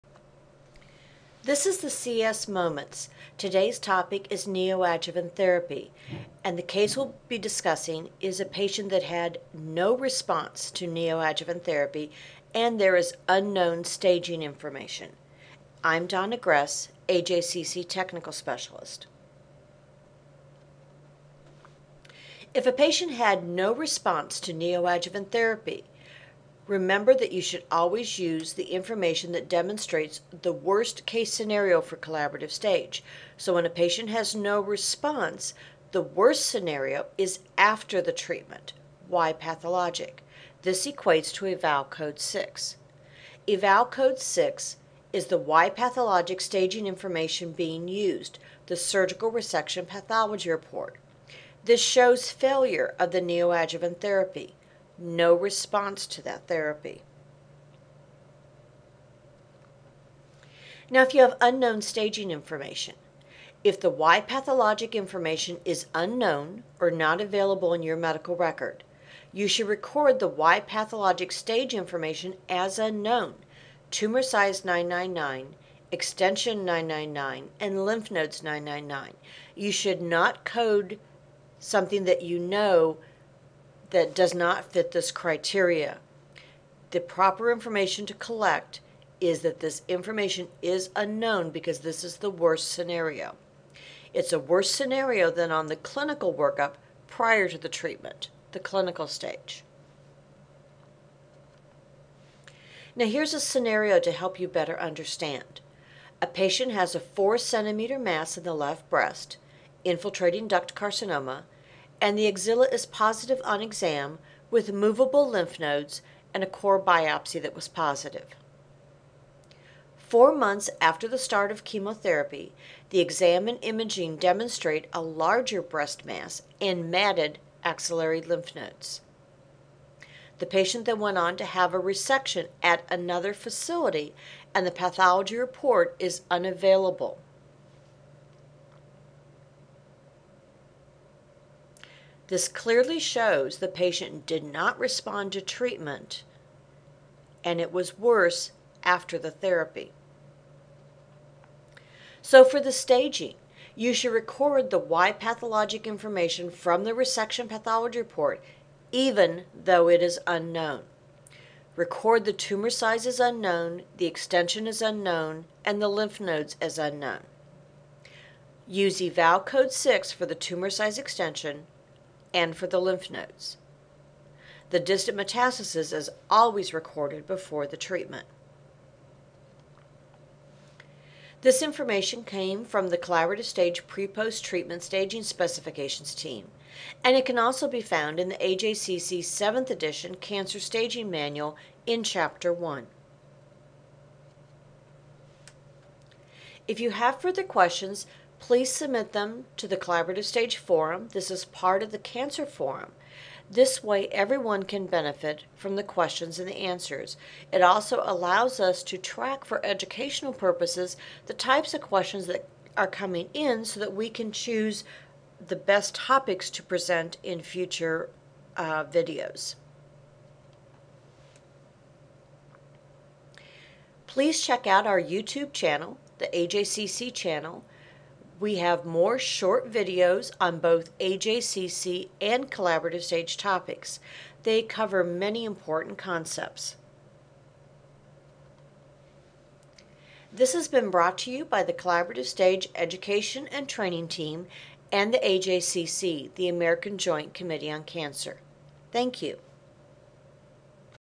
Neoadjuvant Therapy: No Response with Unknown Staging Information
AJCC's Donna Gress presents a scenario in which a patient does not respond to neoadjuvant therapy, and staging information is not available in the medical record.

This presentation was supported by the Cooperative Agreement Number 5U58DP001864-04 from The Centers for Disease Control and Prevention.  Its contents are solely the responsibility of the authors and do not necessarily represent the official views of The Centers for Disease Control and Prevention.